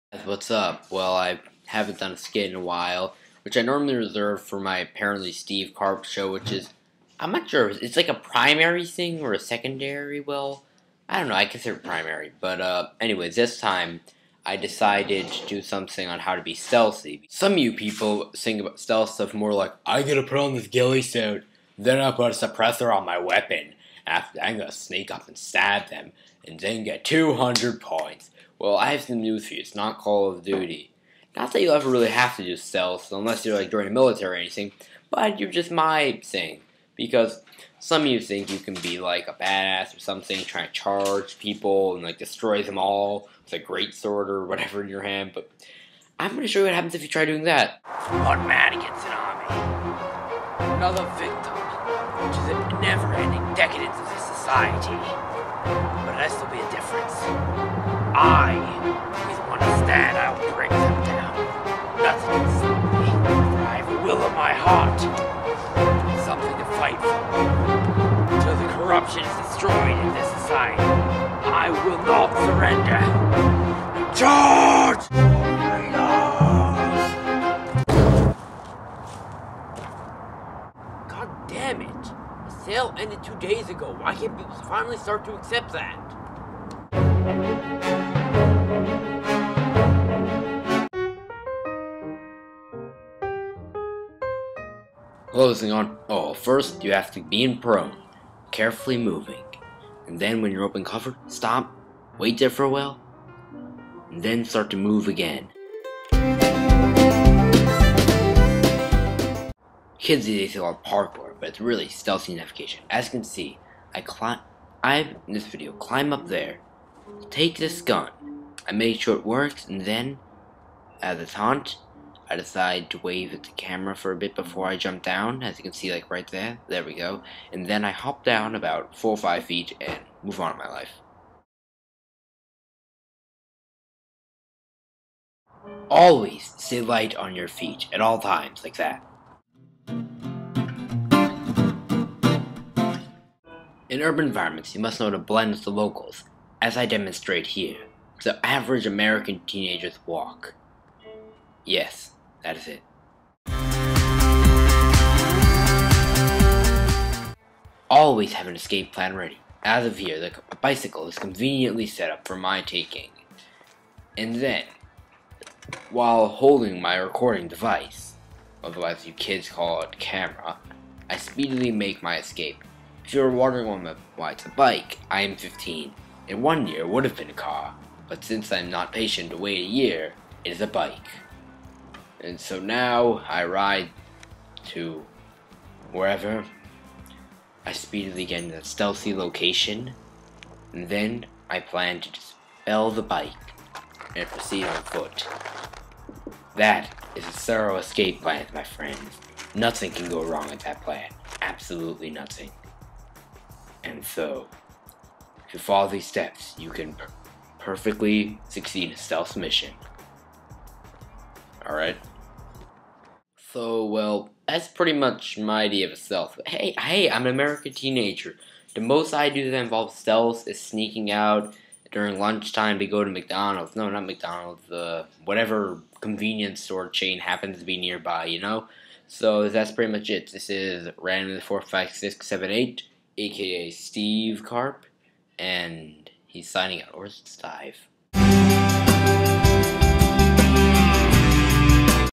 Styve Karp Show Episode 8: How to Be Stealthy
So I finally made Episode 8 of the series with definite end in sight this is just me trying to show you guys how to be stealthy (succeeding or failing at this is your guys call)